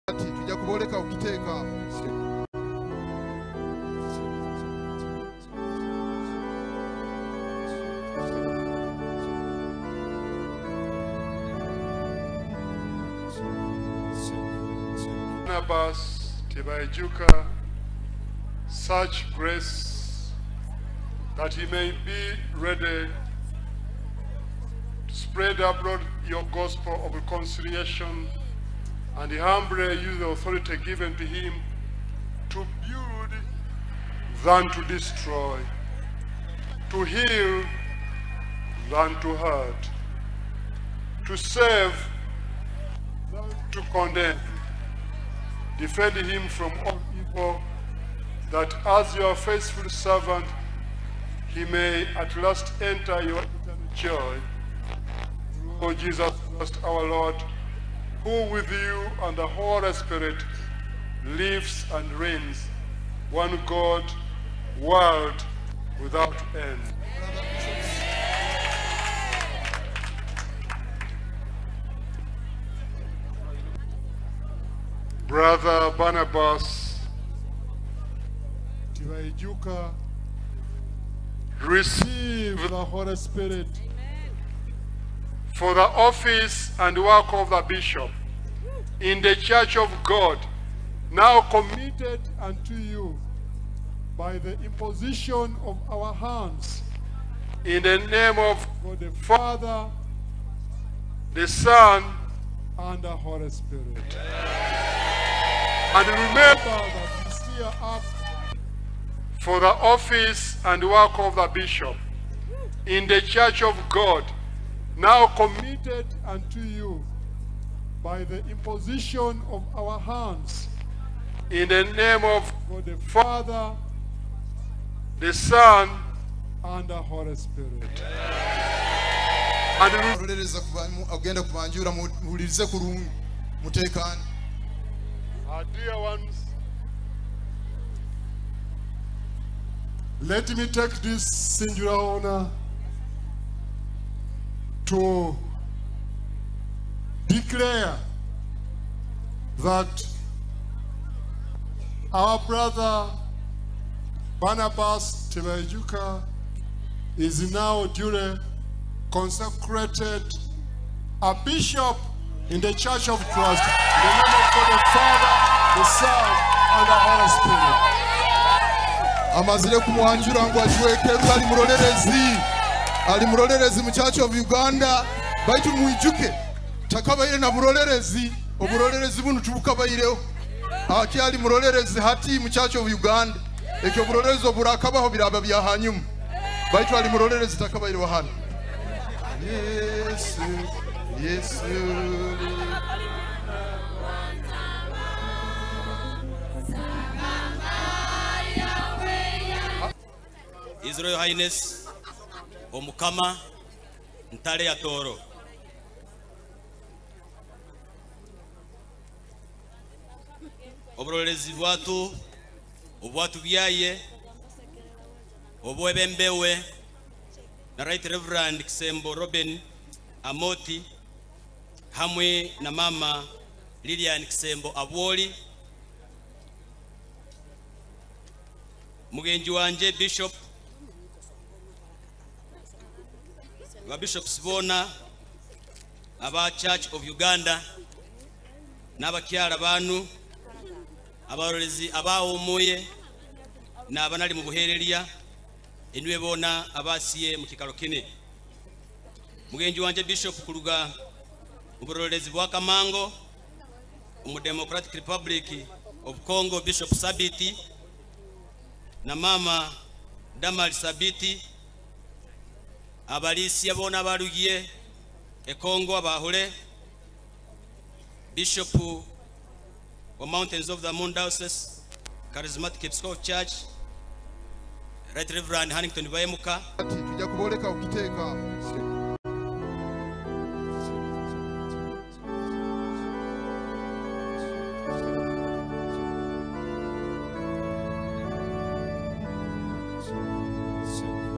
Consecration and enthronement of Rt. Rev. Barnabas Tibaijuka as Bishop of West Ruwenzori
It was joy all over Bundibugyo as thousands of Christians witnessed Rt's official consecration and enthronement. Rev. Barnabas Tibaijuka was the first bishop of the new West Ruwenzori Diocese at St. Barnabas Cathedral in Bundibugyo.
The Archbishop of the Church of Uganda, Dr. Samuel Stephen Kaziimba Mugalu was the principal celebrant at the service and assured Bishop Tibaijuka of prayers and other support in this ministry.